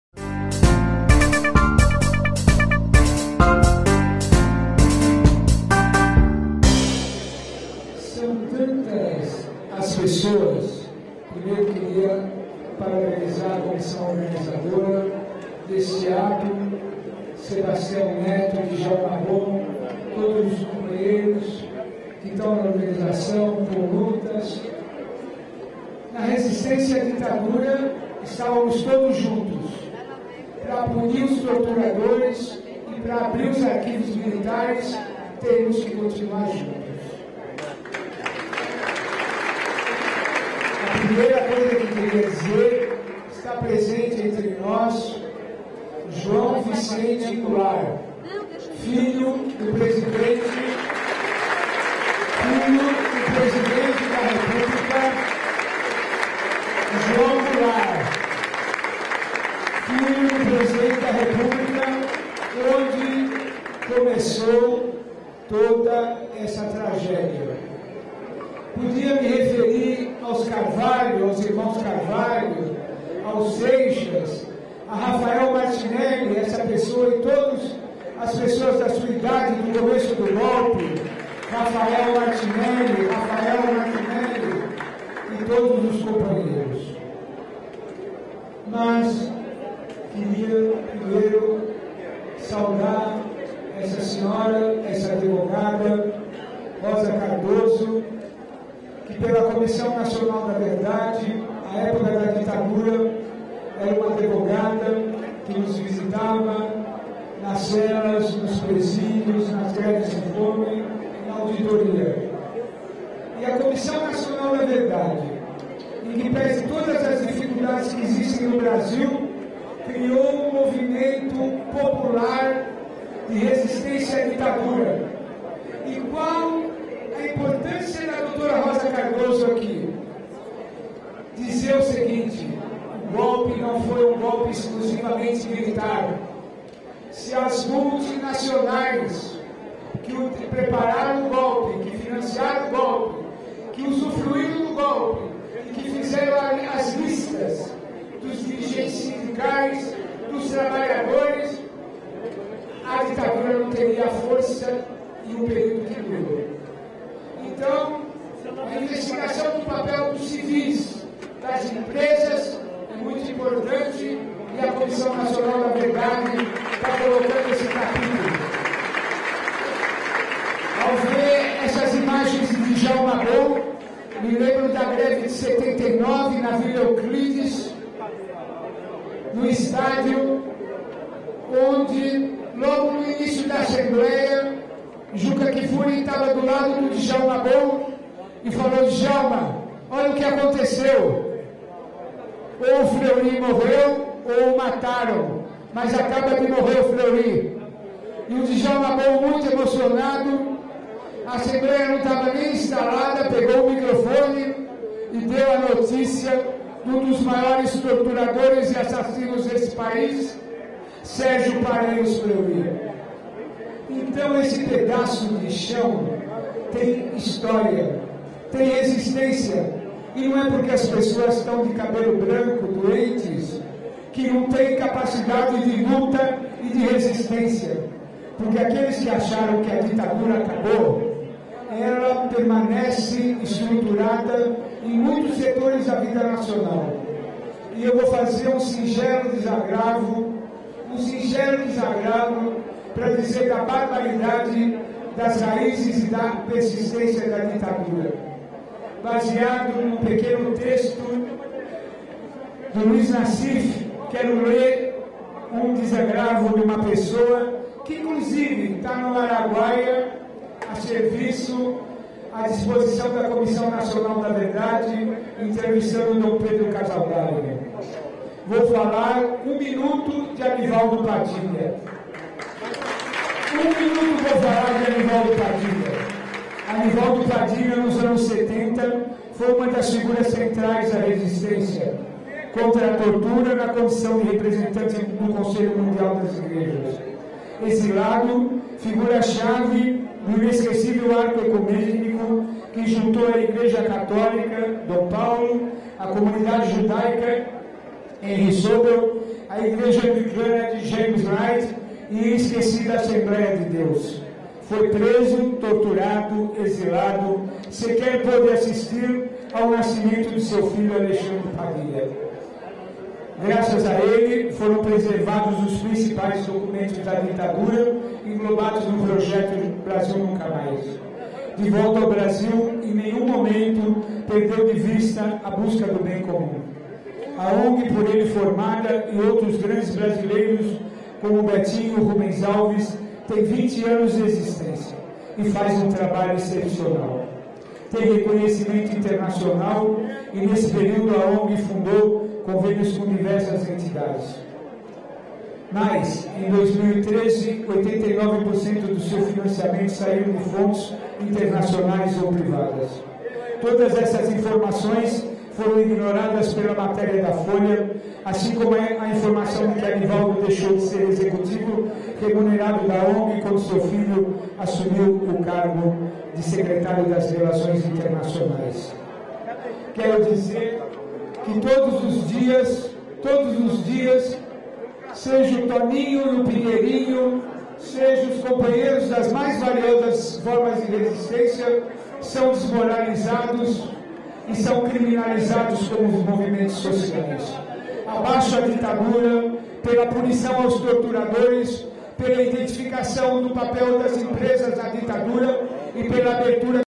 Ato Sindical "Unidos, Jamais Vencidos" - Adriano Diogo
O presidente da Comissão da Verdade do Estado de São Paulo "Rubens Paiva" , deputado estadual Adriano Diogo, em seu discurso, fez uma homenagem ao colaborador da CNV Anivaldo Padilha.

Data: 01/02/2014

Edição: Thalita Rosemberg (CNV)
Vinheta: Thiago Dutra Vilela (CNV)
Arte do canal: Paula Macedo e Isabela Miranda (CNV)
Captação de imagens e áudio: Empresa Brasil de Comunicação (EBC)